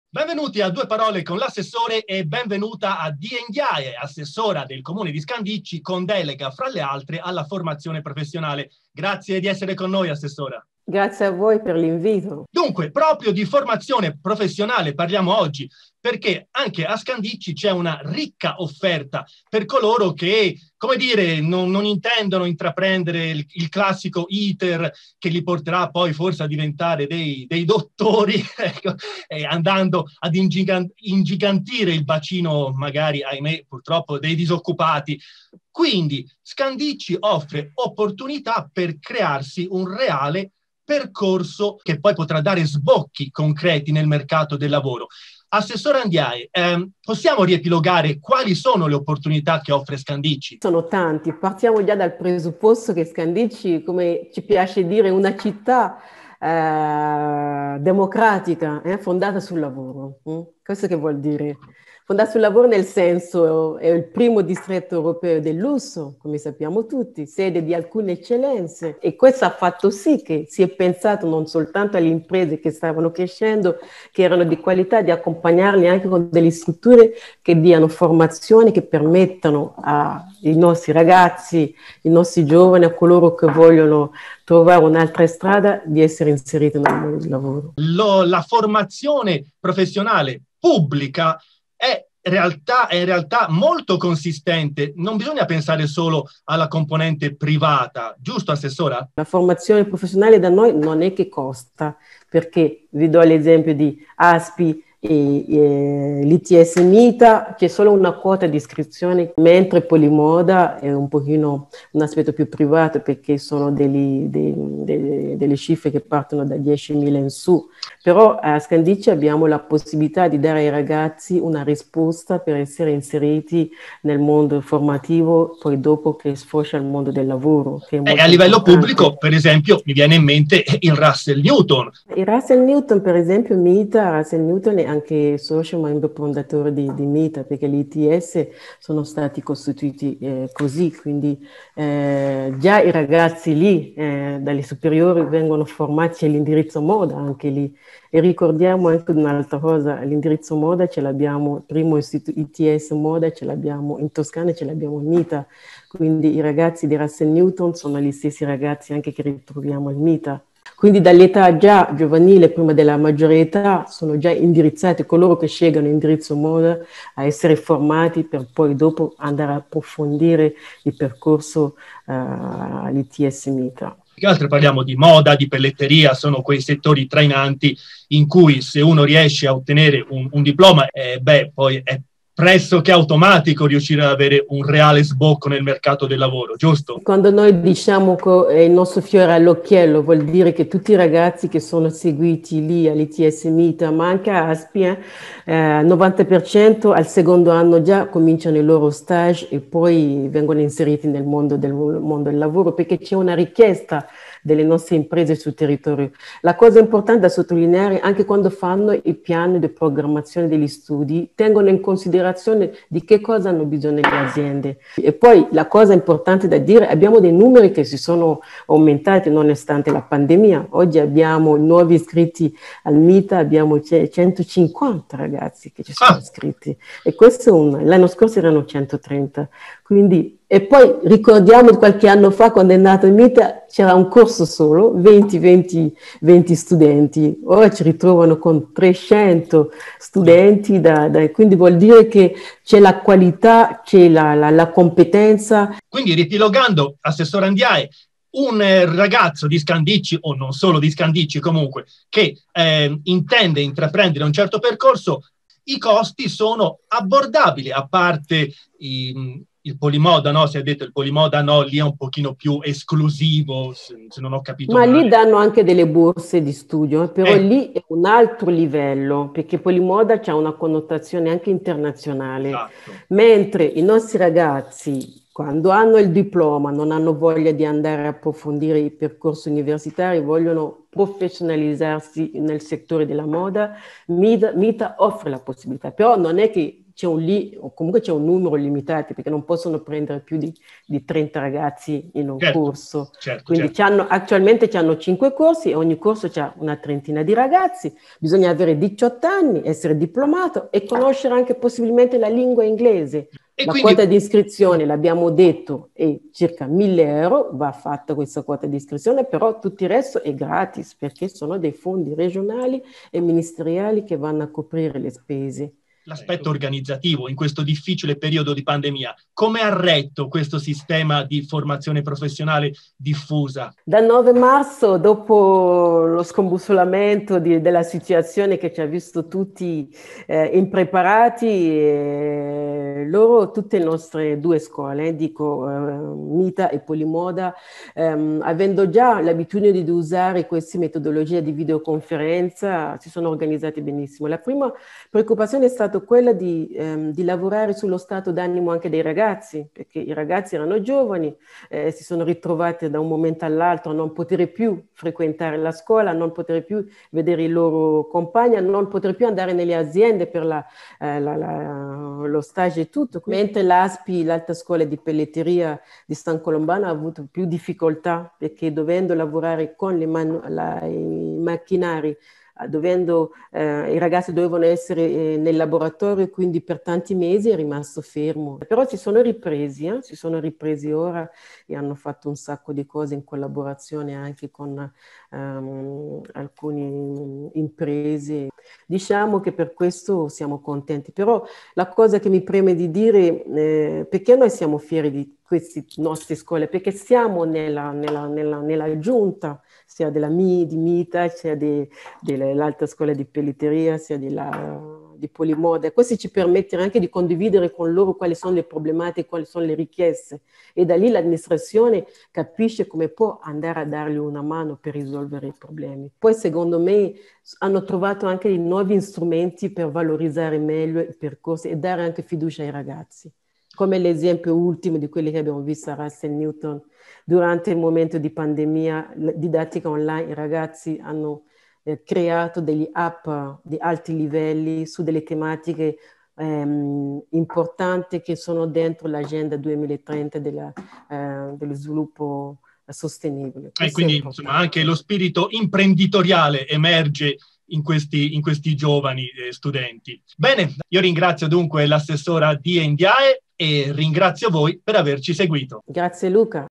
DUE PAROLE CON L'ASSESSORE - Diye Ndiaye - La formazione professionale
La formazione professionale rappresenta una preziosa opportunità di trovare lavoro una volta concluso il percorso di studi. Qual è la situazione a Scandicci? Ne parliamo con l'assessora Diye Ndiaye.